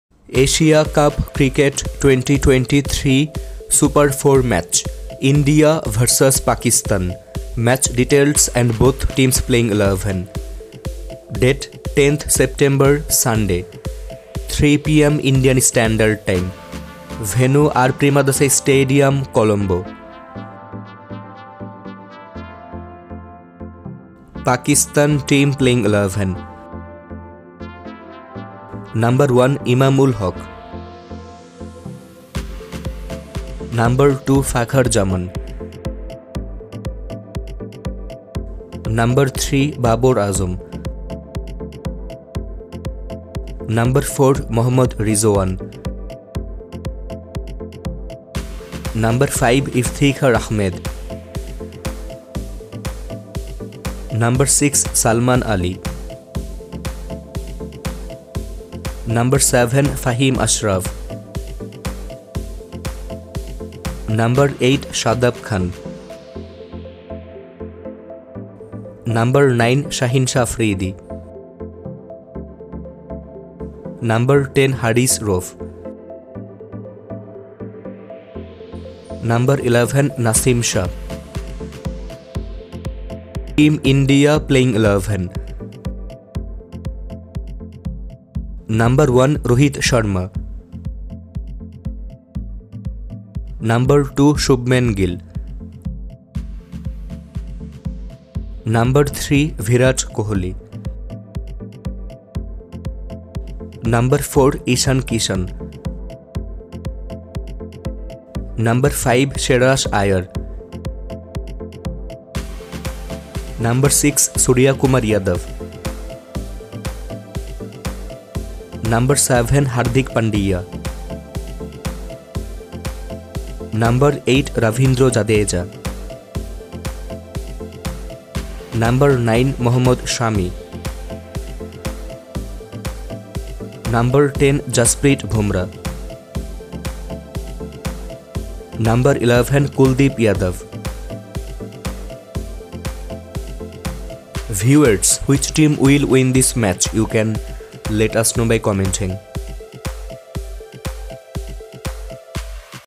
Asia Cup 2023 | Super-4 | India vs Pakistan Match Playing 11
Start: 3:30 PM
Venue: Colombo

1. Rohit Sharma (Captain)
2. Shubman Gill
3. Virat Kohli
4. KL Rahul
5. Shreyas Iyer
6. Hardik Pandya
7. Surya Kumar Yadav
8. Ravindra Jadeja
9. Mohammed Shami
10. Jasprit Bumrah
11. Kuldeep Yadav

1. Imamul Haq
2. Fakhar Zaman
4. Mohammad Rizwan
5. Salman Ali
6. Ifthikhar Ahmed
7. Shadab khan
8. Faheem Ashraf
9. Haris Rauf
10. Shaheen Afridi
11. Naseem Shah